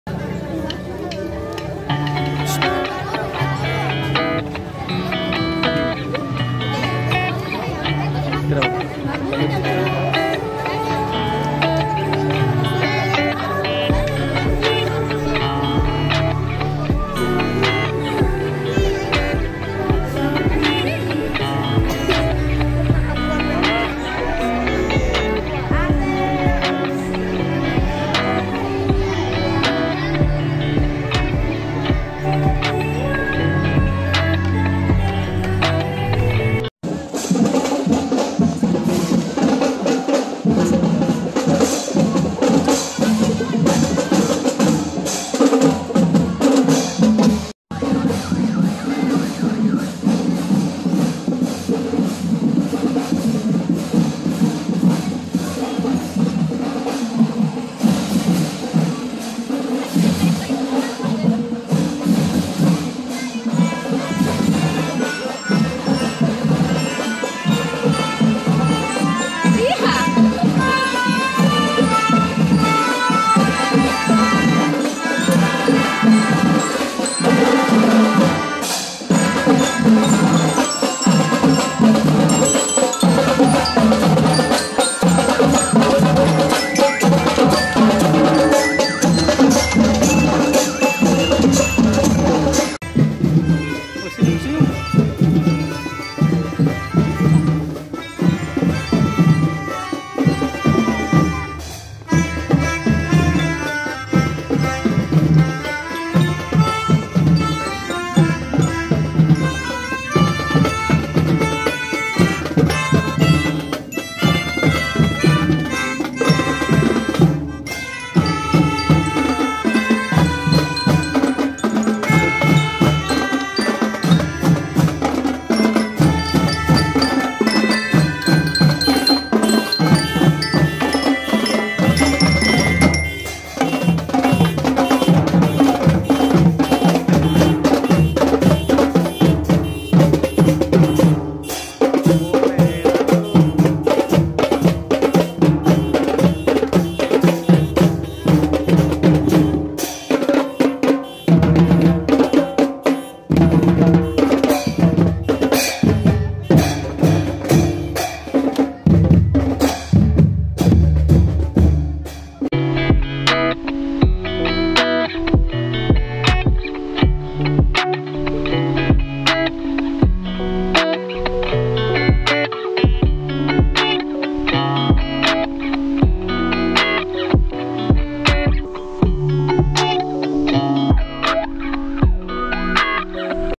marching band min 2 kabupaten Gorontalo di hari hab kemenag kabupaten Gorontalo
hai teman-teman,aku dan teman teman sekolahku menampilkan marching band pada hari hab kemenag kabupaten Gorontalo.. di tonton sampai habis yaa